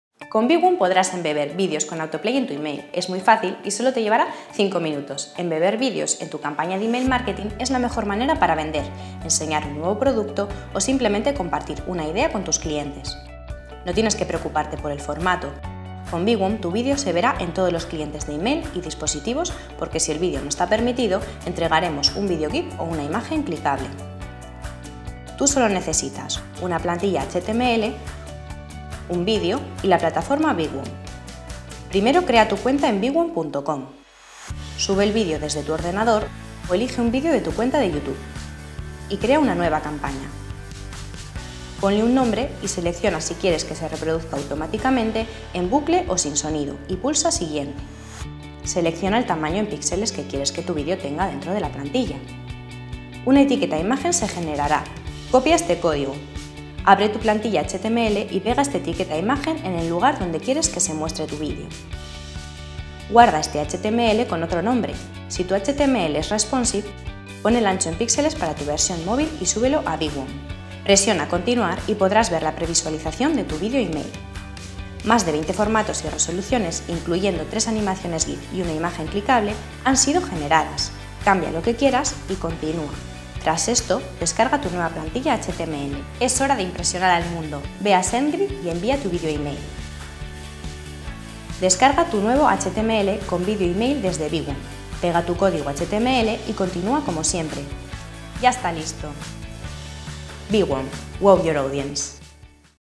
Cómo crear una campaña de vídeo email marketing con SendGrid y Viwom
¿Quieres aumentar tus ventas? Prueba a incluir en cada email que envíes, un vídeo 100% compatible con CUALQUIER DISPOSITIVO, utilizando la tecnología de Viwom (www. viwomail.es), y enviándolo a través de la plataforma digital de SendGrid.

Es muy fácil, solo sigue las instrucciones de este vídeo, y haz que tus clientes no solo te lean ¡Sino que también te vean!

Incrusta en tu email un vídeo que SE REPRODUZCA DE FORMA INMEDIATA con tan solo abrir el correo electrónico, y aumenta tu visibilidad, mejora hasta el 50% tus ventas y alcanza hasta un 280% en tu CTR (tasa de clics).